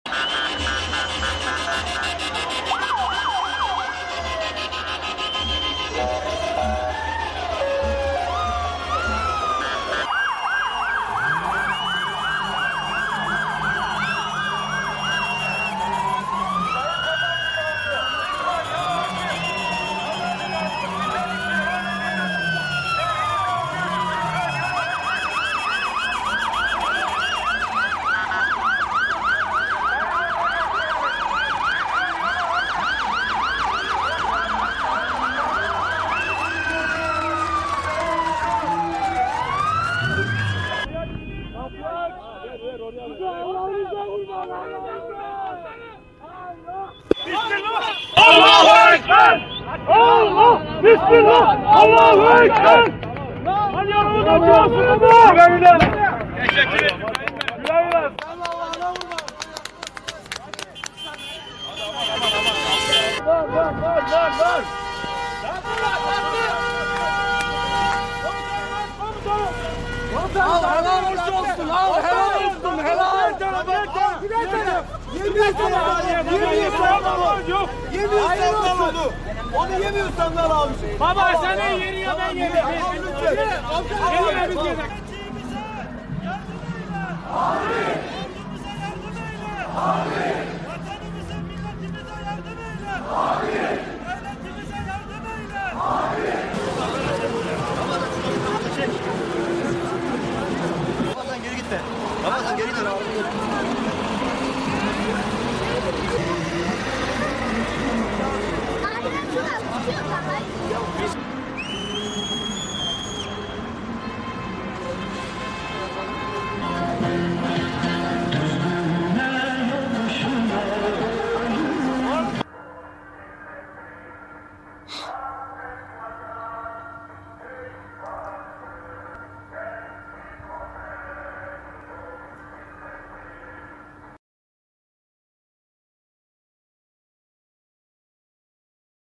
Kahramanmaraş´tan Mehmetçiğe sevgi seli
Kahramanmaraş’ta, Zeytin Dalı Operasyonu için sınır hattına giden askeri konvoya vatandaşlar, Pazarcık ilçe merkezinde uğurlama töreni düzenledi.